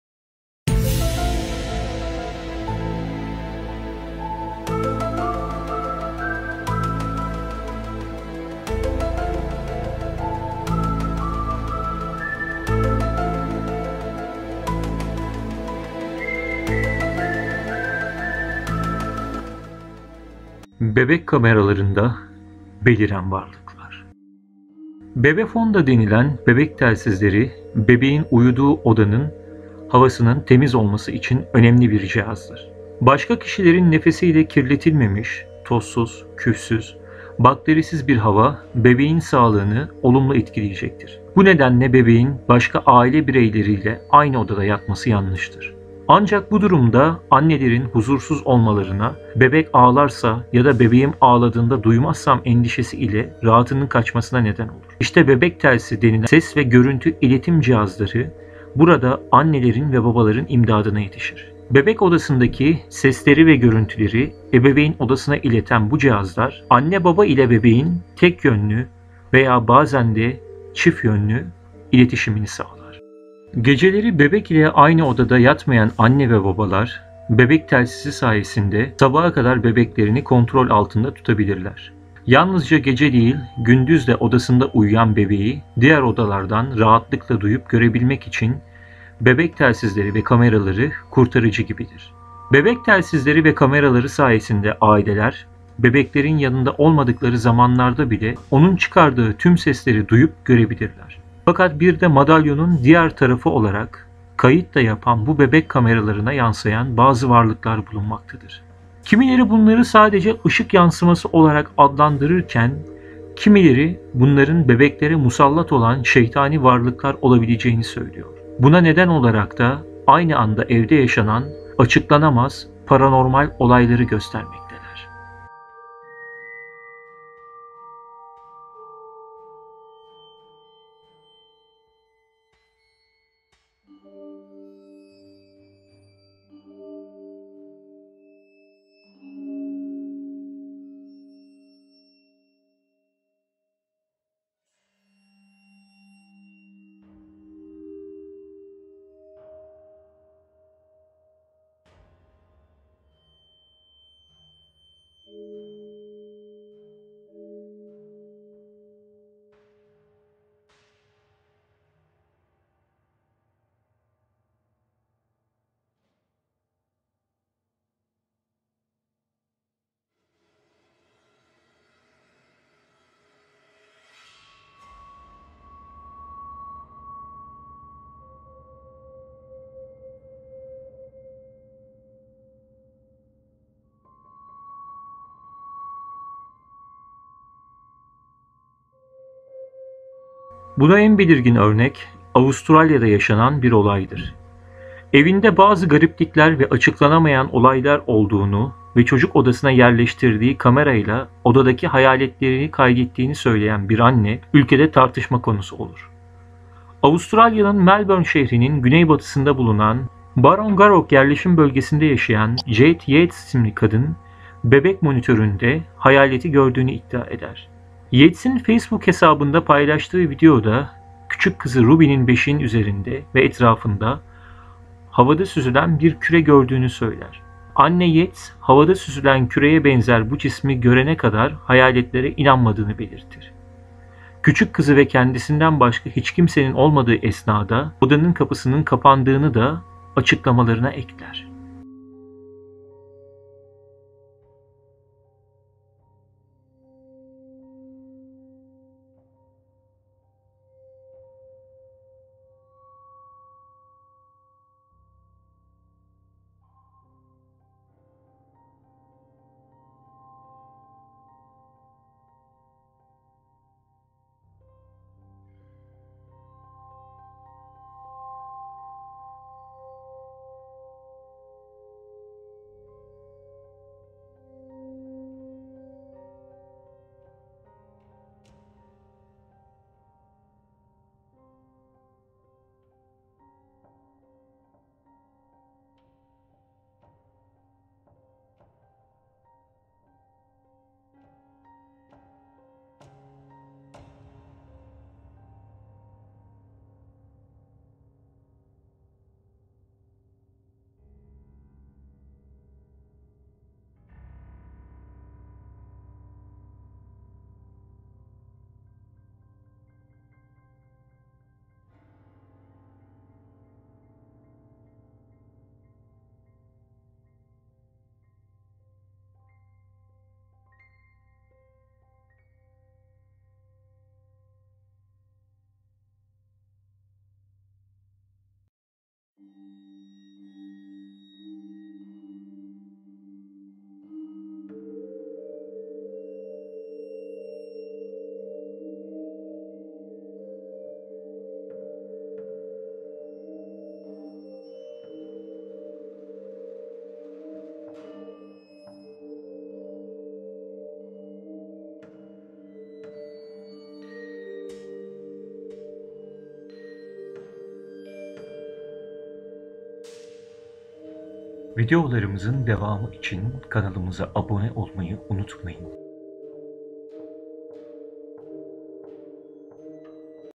Bebek Kameralarında Beliren Varlıklar
Geceleri, bebek ile aynı odada yatmayan anne ve babalar, bebek telsizi sayesinde sabaha kadar bebeklerini kontrol altında tutabilirler. Yalnızca gece değil, gündüz de odasında uyuyan bebeği diğer odalardan rahatlıkla duyabilmek için bebek telsizleri kurtarıcı gibidir. Bebek telsizleri ve kameraları  sayesinde aileler, bebeklerinin yanında olmadıkları zamanlarda bile onun çıkardığı tüm sesleri duyup görebilirler.Fakat birde madalyanon diğer tarafı olarak. Kayıtda yapan bu bebek kameralarına yansıyan bazı varlıklar bulunmakta. Kimileri bunları sadece ışık yansıması olarak adlandırırken. Kimileri bunların bebeklere musallat olan şeytani varlıklar olabileceğini söylüyor, buna neden olarakta aynı anda evlerde yaşanan açıklanamaz paranormal olayları göstermekteler .